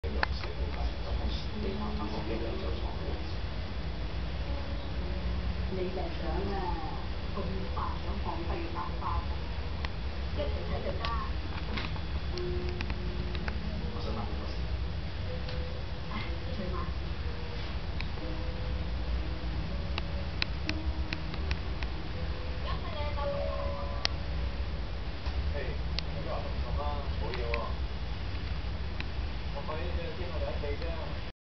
Bunny grooming
Mau Mau is grooming BB